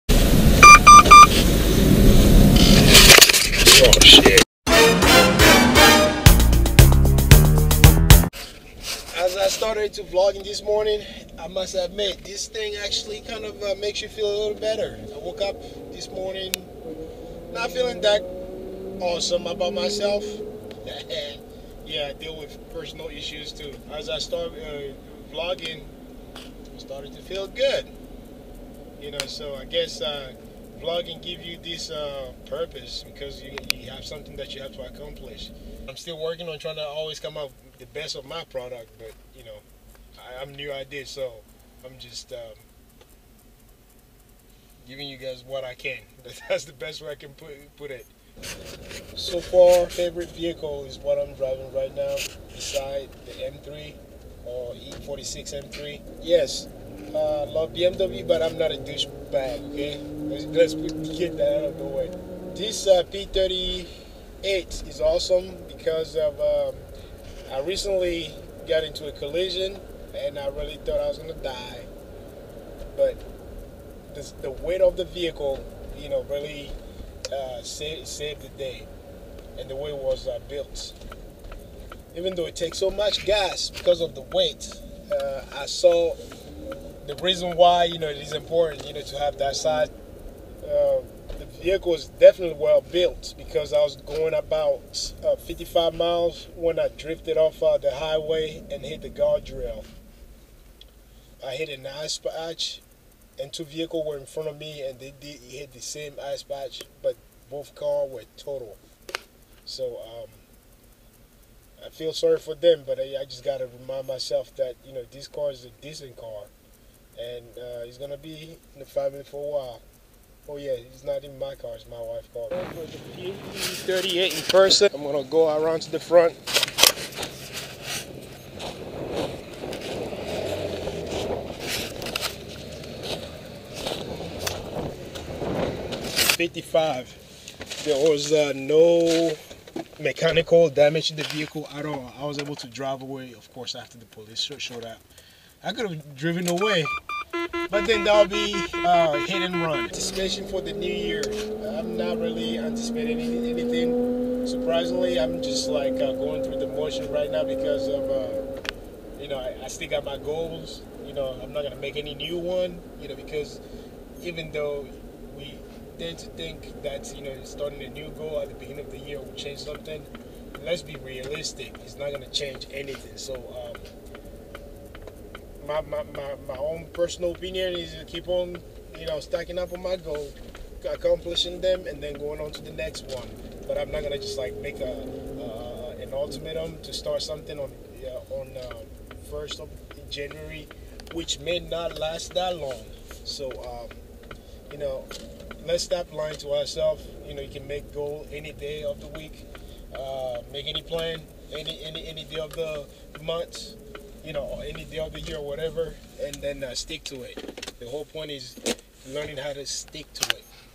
Landrover P38, a tank on wheel
I got into an accident going at 55 mph with minimal damage to the Land Rover P38.